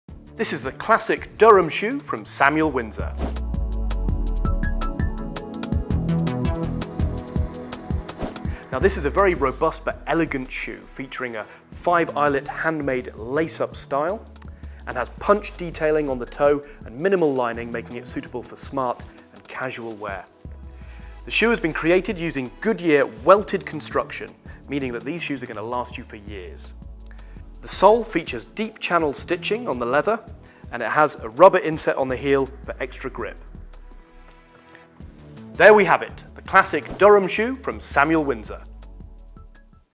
Classic Durham shoe in Brown from Samuel Windsor
The Durham shoe offers the unique combination of unrivalled comfort and rugged wearability. Made with a goodyear welted construction for maximum comfort and a lifetime of good looks and reliable wear, the Durham works well with a suit for work or with weekend casual attire. Provided with a shoehorn and spare laces.

Available in sizes: 5, 6, 6½, 7, 7½, 8, 8½, 9, 9½, 10, 10½, 11, 12, 13, 14.